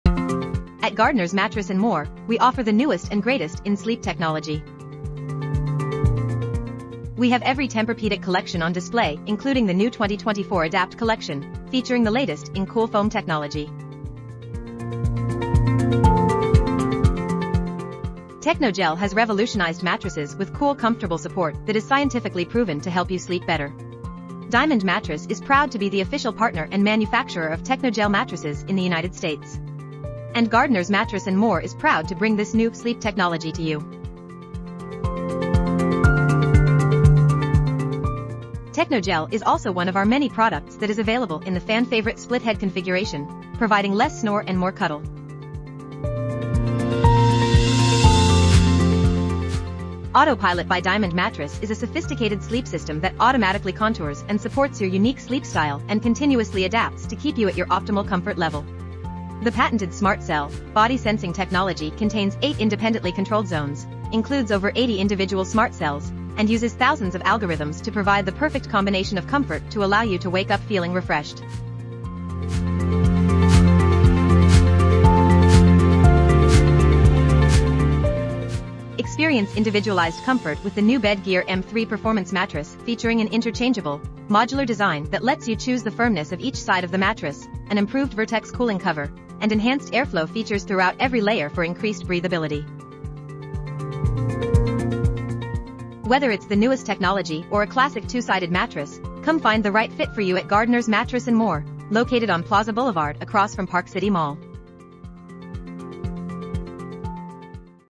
🛏️ Discover the latest in sleep technology at Gardner's Mattress and More! 🌙
Gardner's Mattress & More
830 Plaza Blvd (Behind Park City Mall)

Explore every Tempur-Pedic collection, including the 2024 Adapt with Cool Foam tech.

Experience Technogel's cool comfort and Diamond Mattress' innovative sleep systems.

Enjoy individualized comfort with Bedgear M3's modular design.